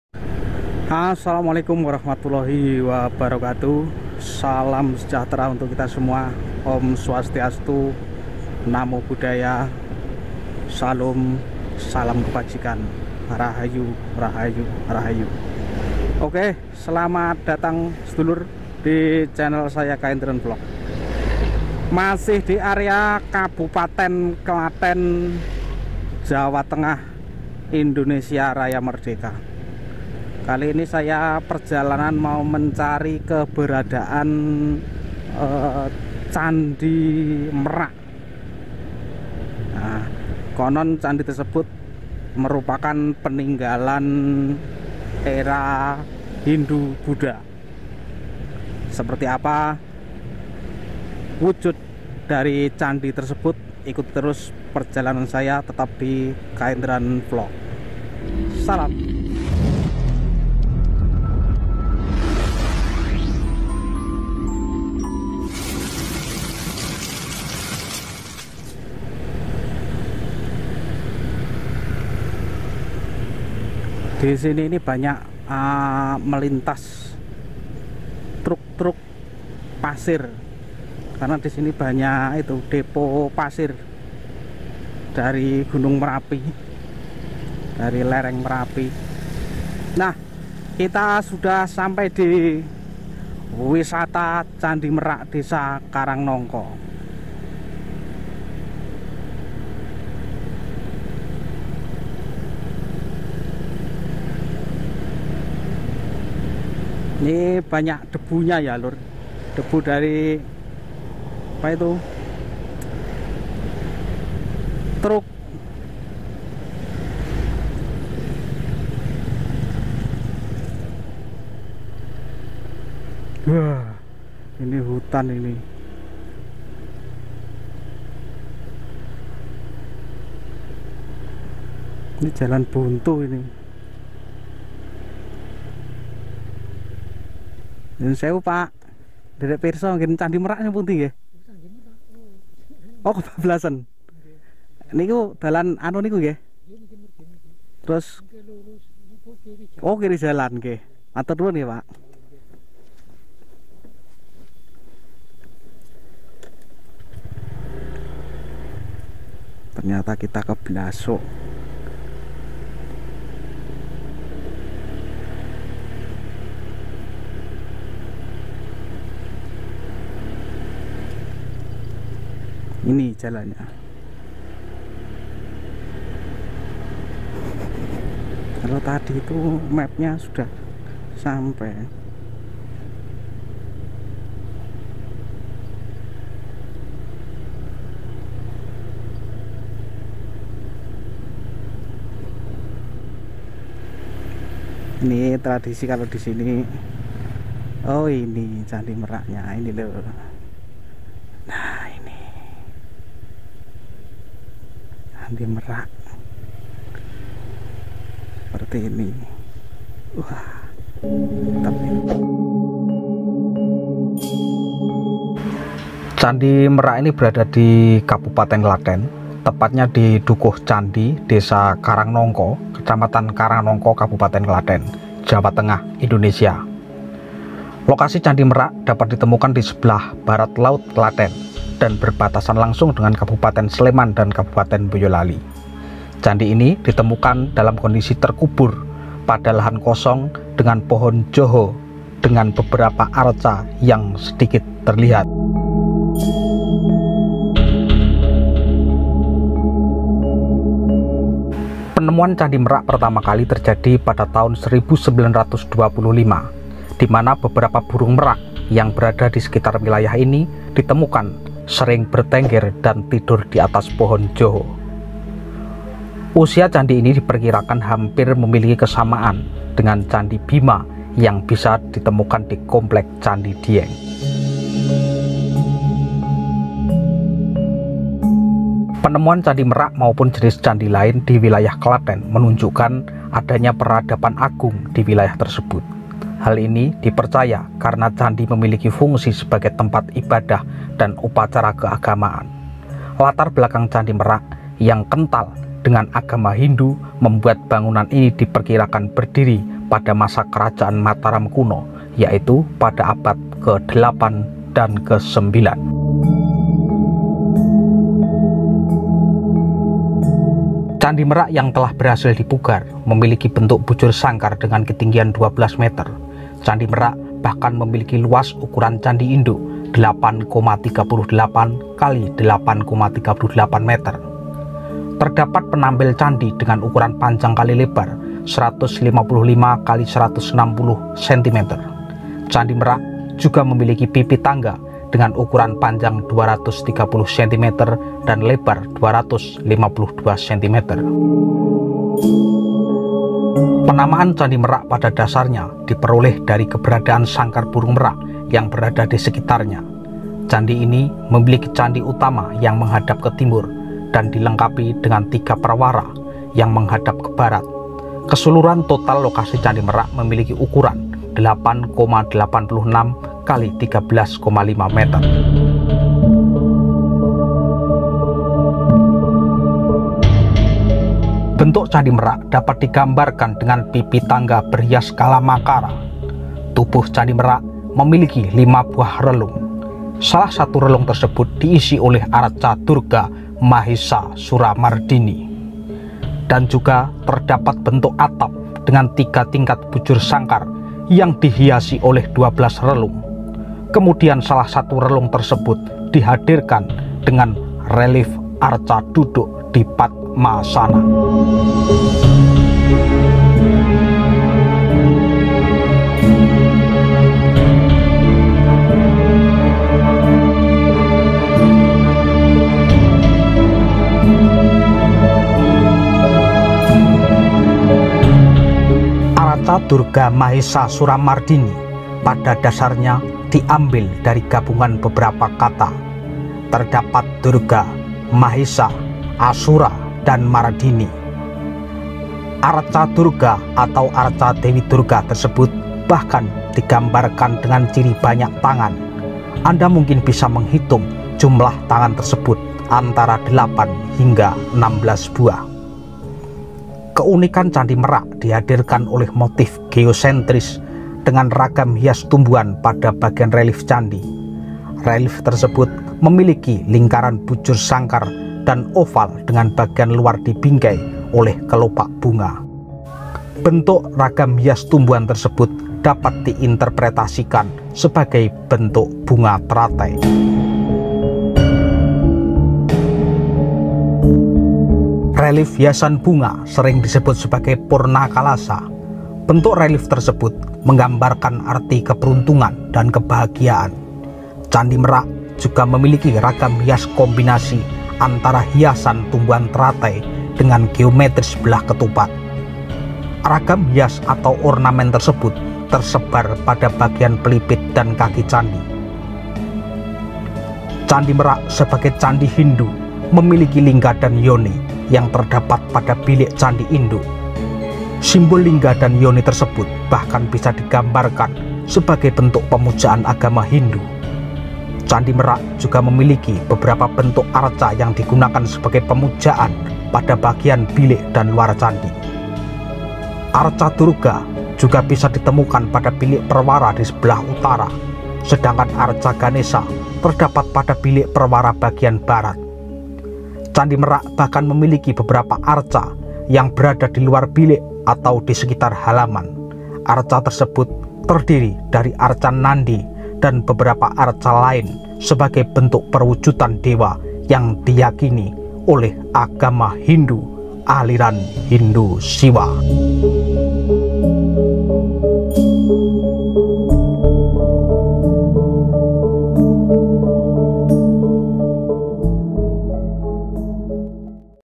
Inilah Awal Mula Ditemukannya Candi Merak Klaten
❤️❤️ WellCome ❤️❤️
  🌻 KAENDRAN VLOG🌻

Candi Merak (Hanacaraka:ꦕꦤ꧀ꦝꦶ​ꦩꦼꦫꦏ꧀, Candhi Merak) adalah peninggalan purbakala berupa kompleks percandian di Dukuh Candi, Desa Karangnongko, Kecamatan Karangnongko, Kabupaten Klaten, Jawa Tengah. Lokasi ini berada di sebelah barat laut dari wilayah Klaten, berbatasan dengan Kabupaten Boyolali dan Kabupaten Sleman.

✅ᴅᴏɴ'ᴛ ꜰᴏʀɢᴇᴛ ᴛᴏ ꜱᴜʙꜱᴄʀɪʙᴇ 🔔